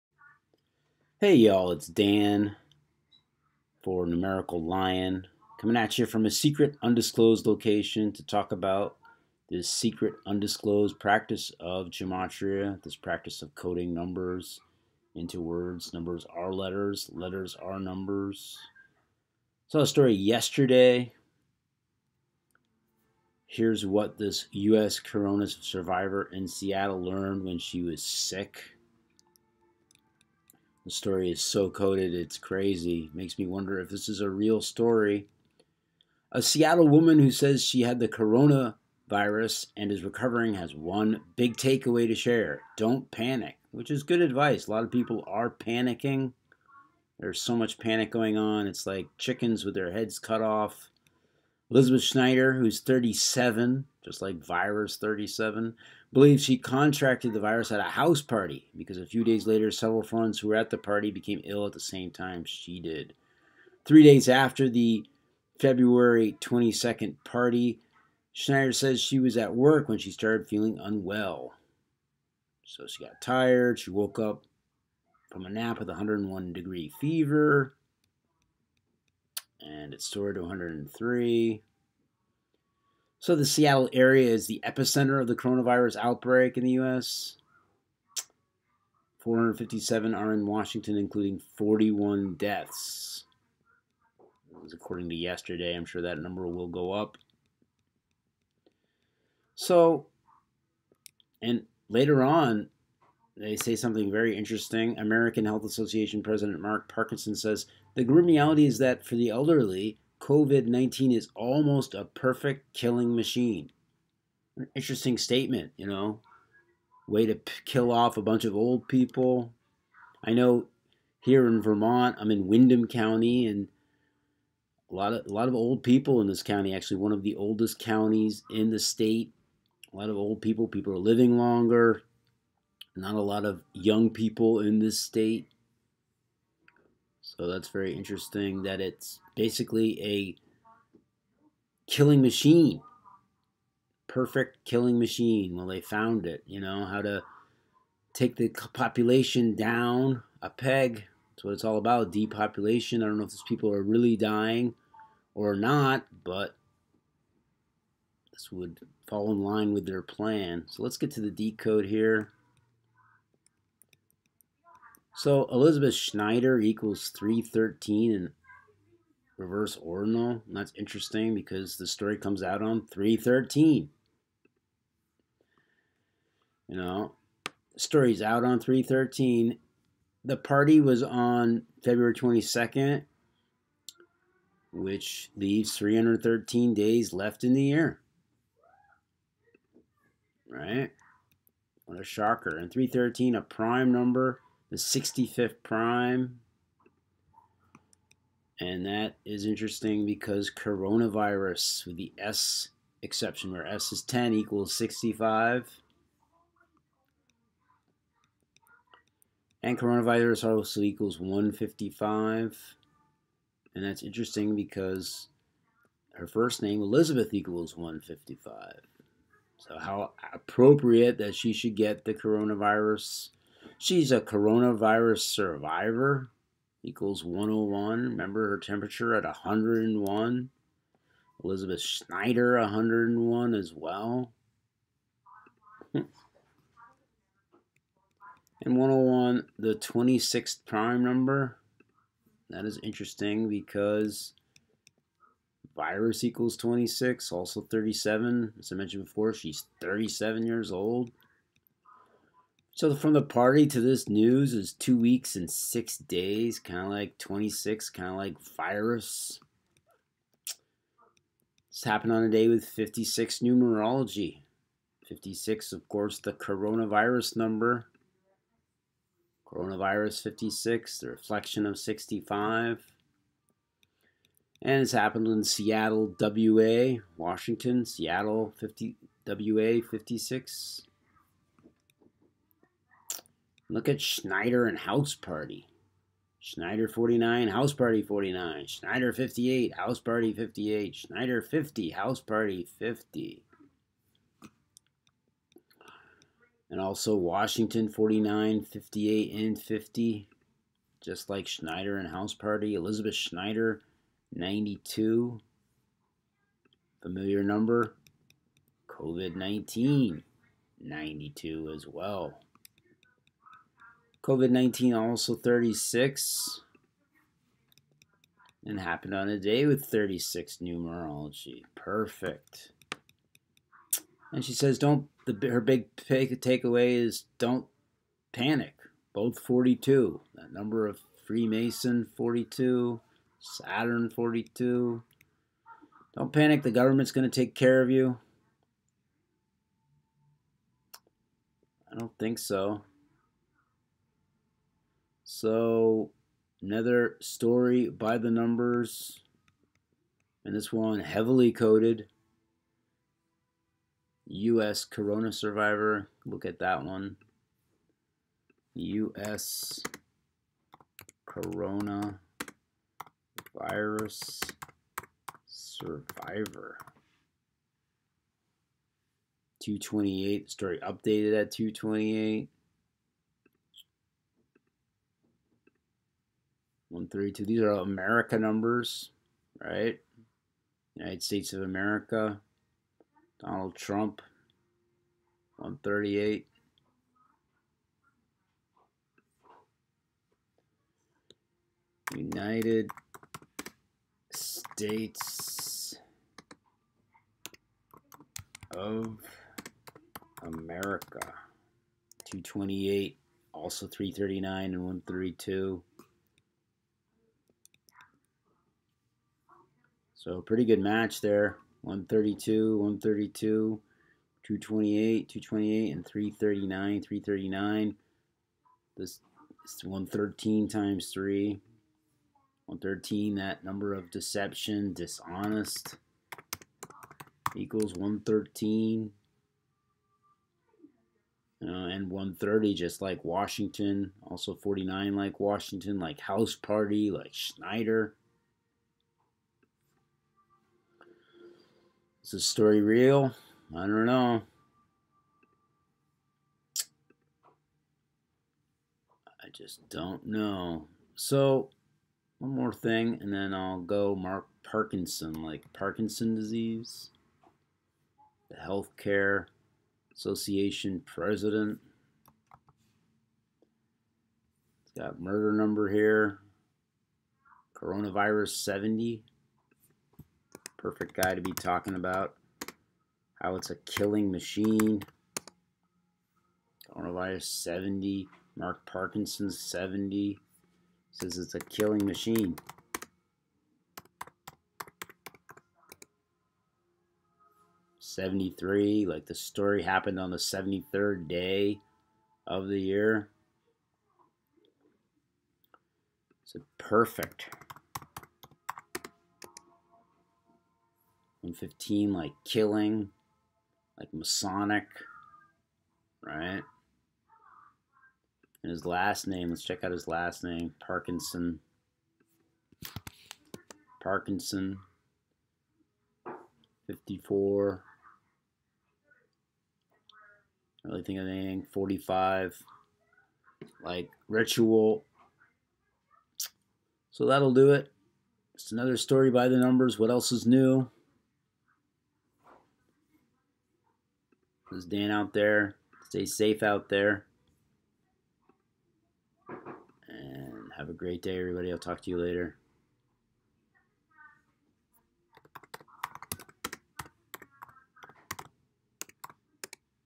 Seattle corona virus survivor says don't panic
Elizabeth Schneider had the corona virus by the code